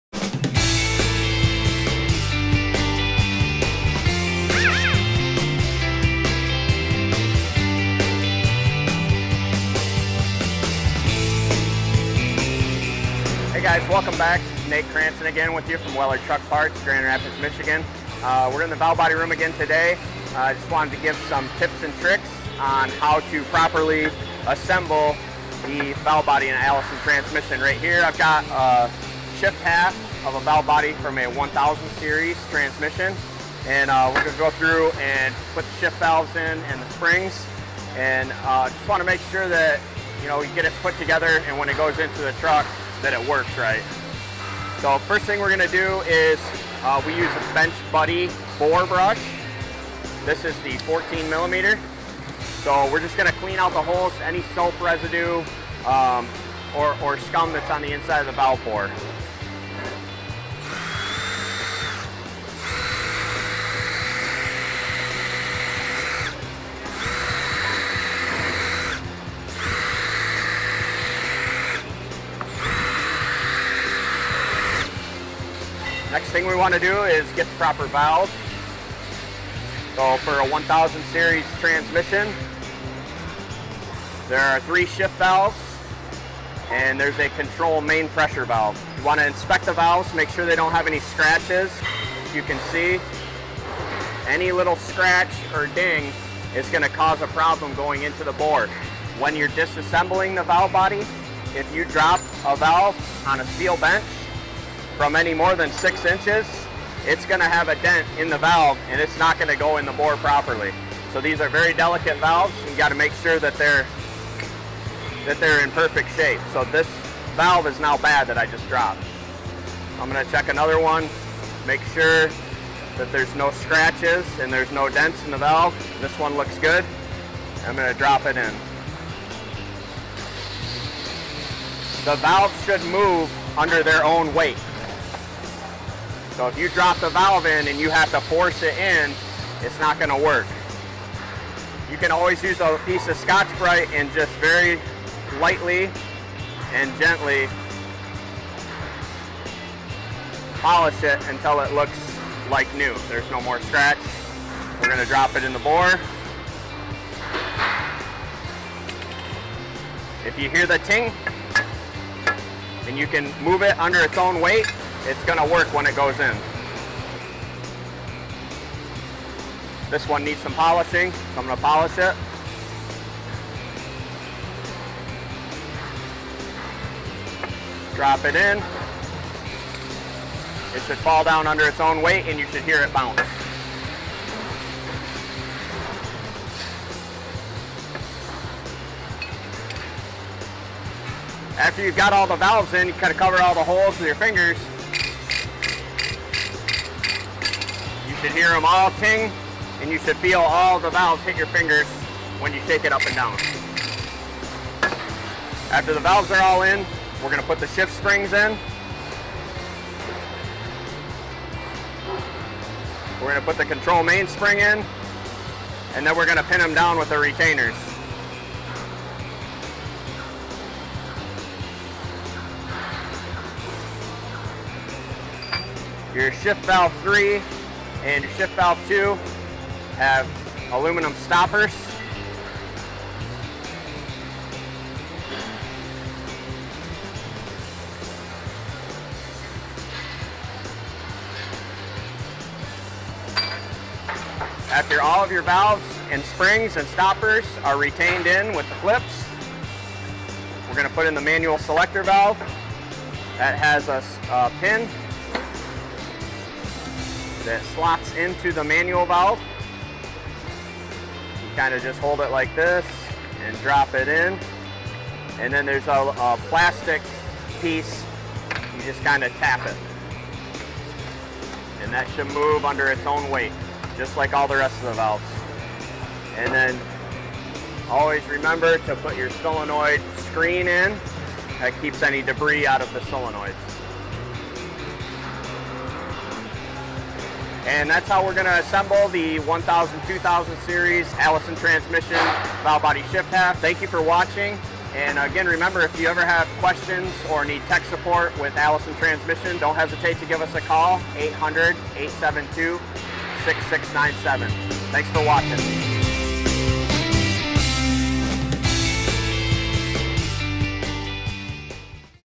Valve Body Assembly
In this video Nate will show us how to assemble a valve body for a 1000/2000 series Allison transmission.